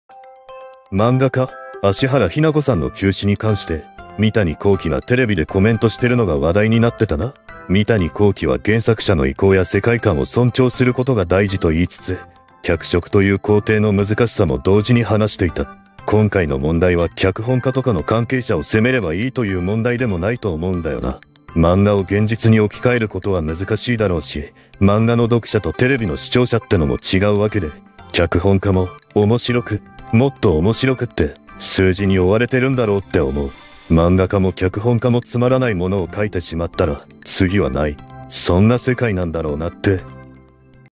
三谷幸喜、セクシー田中さんの件を語る
どうにもならなかったのか…